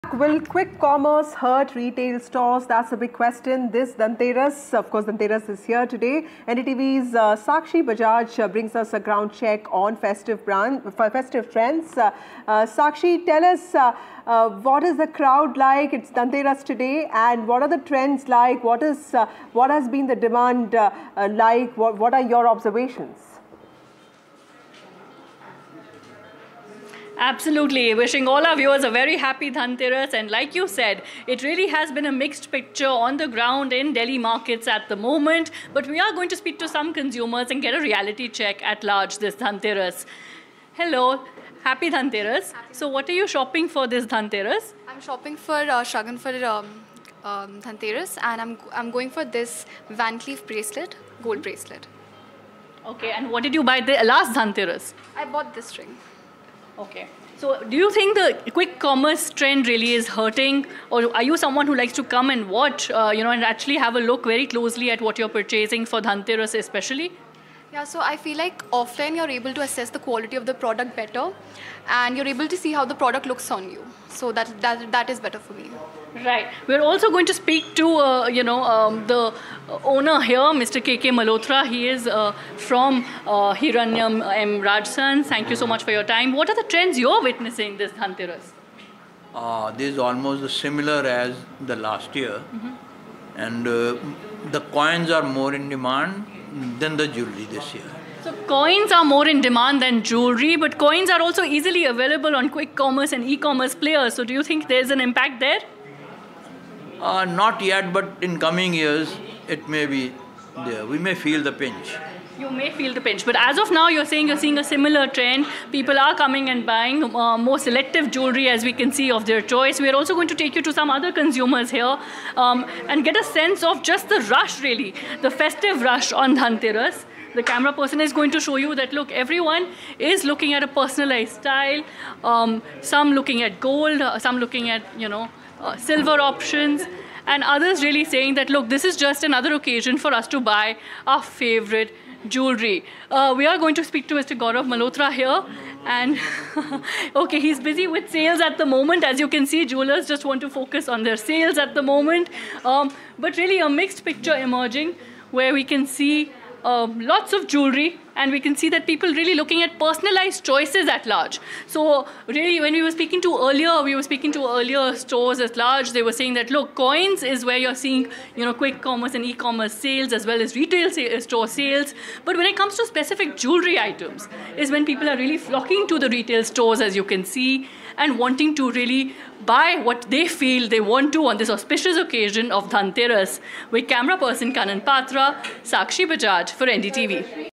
Dhanteras 2024 | Will Quick Commerce Hurt Retail Stores This Dhanteras?
NDTV's Sakshi Bajaj does a ground check on the festive trends this Dhanteras.